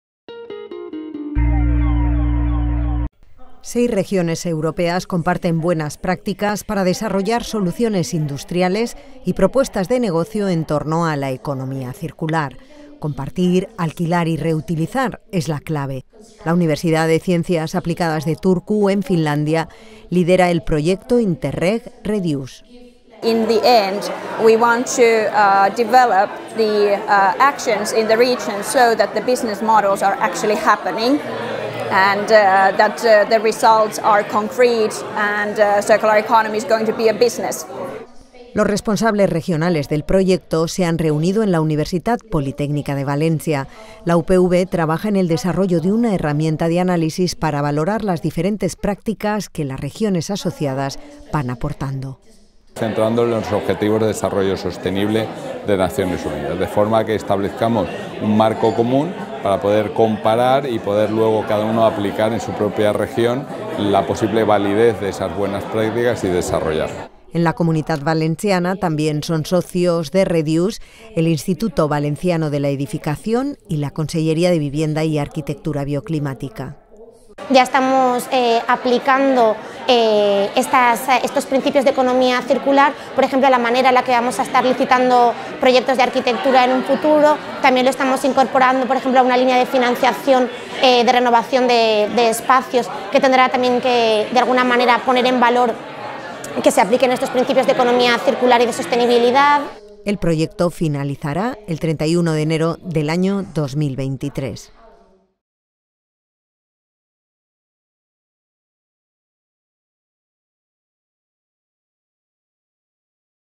Proyecto Reduces: economía circular - Noticia @UPVTV, 11-03-2020
Seis regiones europeas comparten buenas prácticas para desarrollar soluciones industriales y propuestas de negocio en torno a la economía circular. Compartir, alquilar y reutilizar es la clave del proyecto europeo Interreg Reduce. La Universidad de Ciencias Aplicadas de Turku, en Finlandia, lidera el proyecto.

Los responsables regionales del proyecto Intereg Reduces se han reunido en la Universitat Politècnica de València. La UPV trabaja en el dearrollo de una herramienta de análisis para valorar las diferentes prácticas que las regiones asociadas van aportando.

En la Comunitat Valenciana también son socios de Reduce el Instituto Valenciano de la Edficación y la Conselleria de Vivienda y Arquitectura Bioclimática.

El proyecto finalizará el 31 de enero del año 2023.